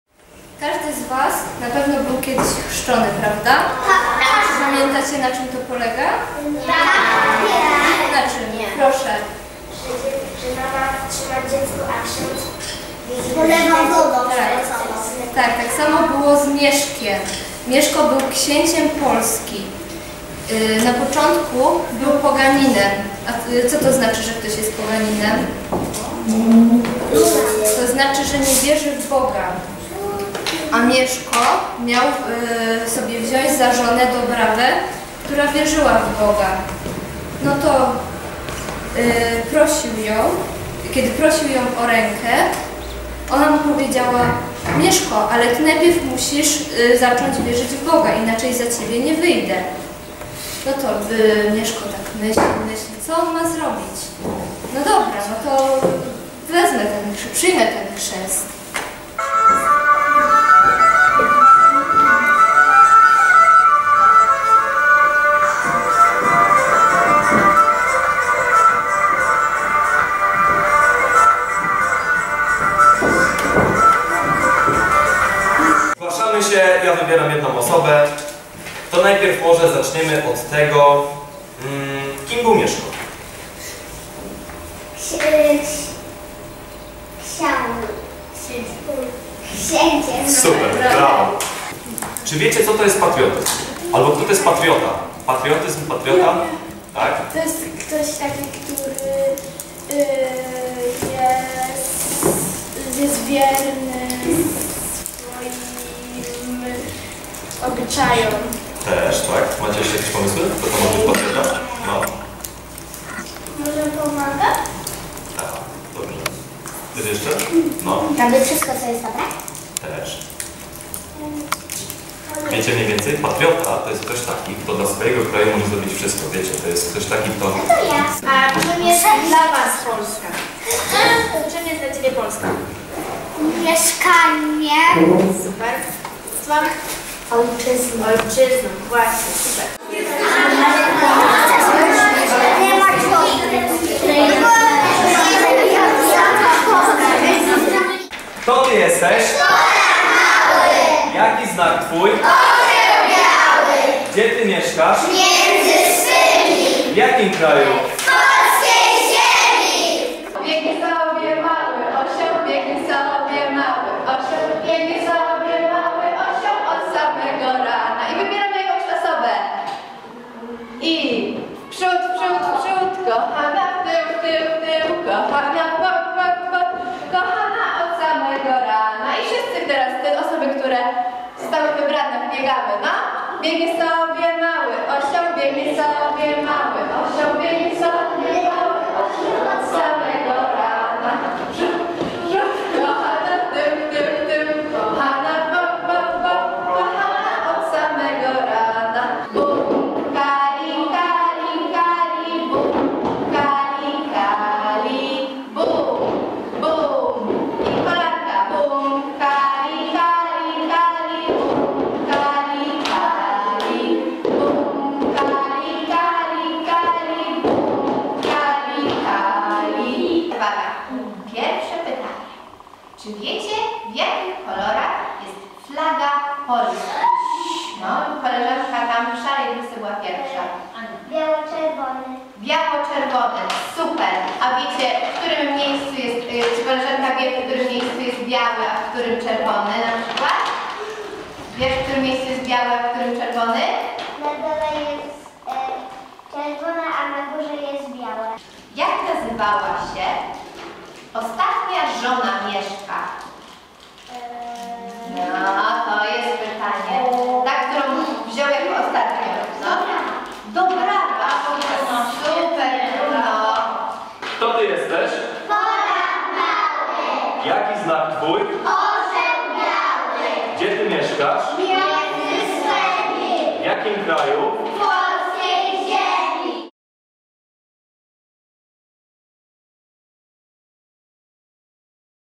VI Liceum Ogólnokształcące im. Ignacego Jana Paderewskiego w Poznaniu - projekt społeczny
„Projekt społeczny.”
Filmik autorstwa uczniów VI Liceum Ogólnokształcącego im. Ignacego Jana Paderewskiego w Poznaniu. Projekt został zorganizowany w ramach Akademii Nowoczesnego Patriotyzmu. Edycja 2015/2016.